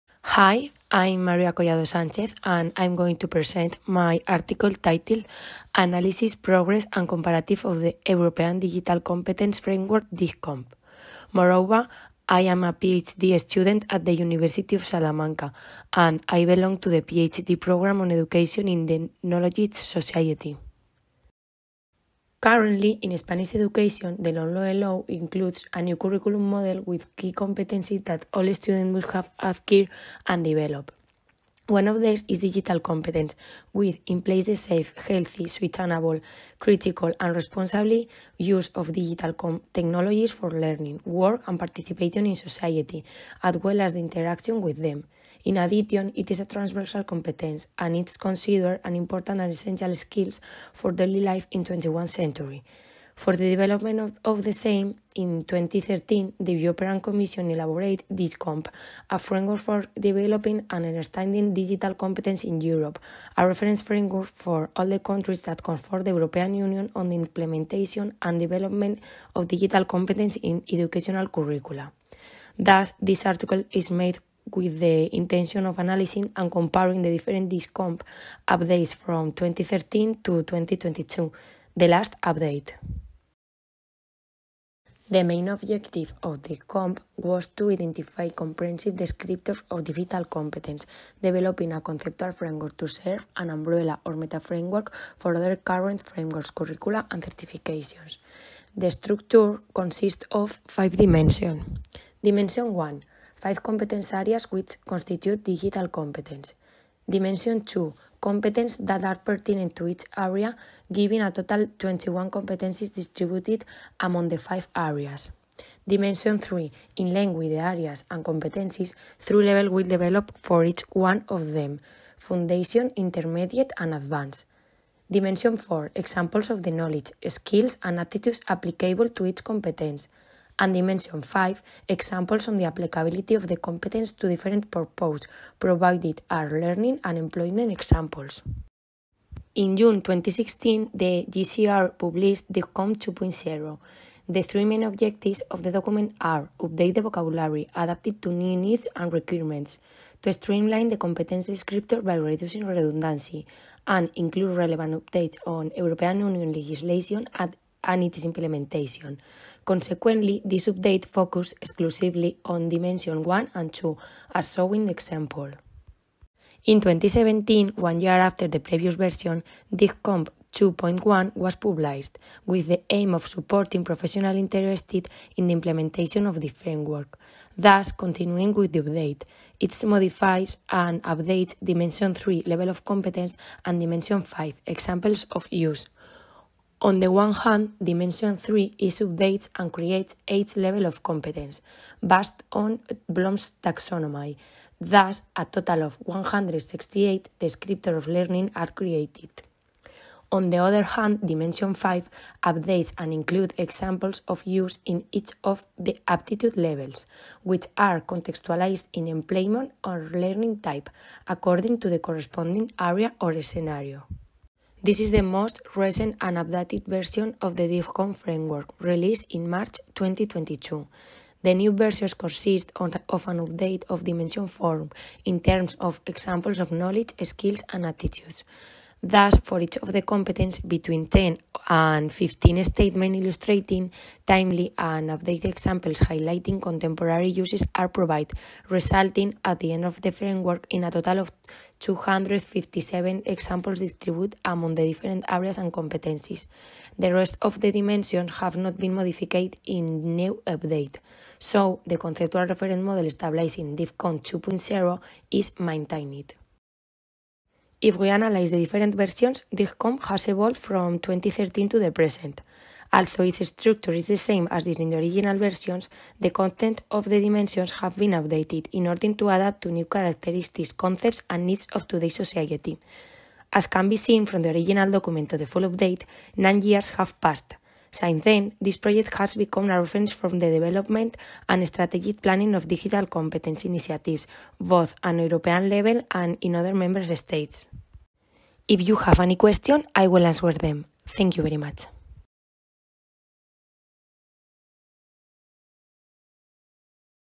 ID033 - Analysis, progress and comparative of the European Digital Competence Framework DIGCOMP
ID033 - Analysis, progress and comparative of the European Digital Competence Framework DIGCOMP. María Collado-Sánchez, Francisco José García-Peñalvo, Ana María Pinto-Llorente.